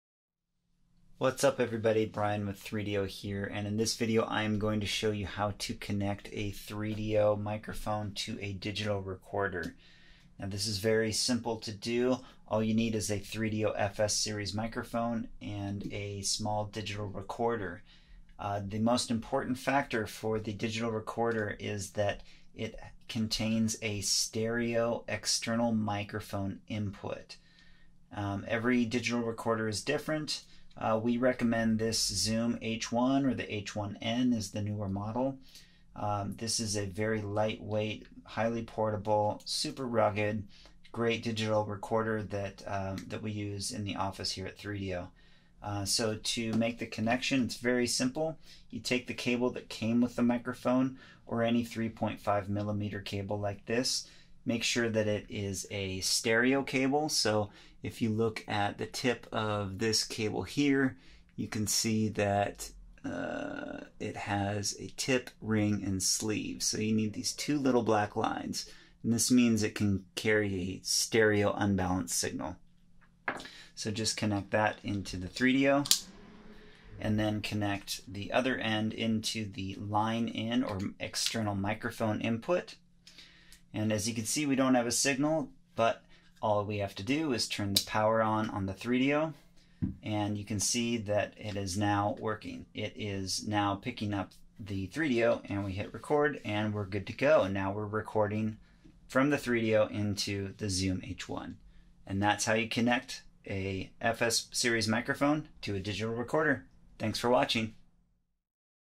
How to Record 3Dio Binaural Audio Using a Zoom H1
To record 3Dio binaural audio into a Zoom H1, you'll need the following:
- A 3Dio Microphone
- A Zoom H1
- A 3.5mm Stereo TRS M2M cable

1) Plug one end of the cable into the 3Dio mic.
2) Plug the other end of the cable into the 'Mic/Line In' of the Zoom H1
3) Turn the 3Dio power switch to the ON position.
4) Turn the Zoom H1 power switch to the ON position.
5) Plug your headphones into the headphone output of the Zoom H1.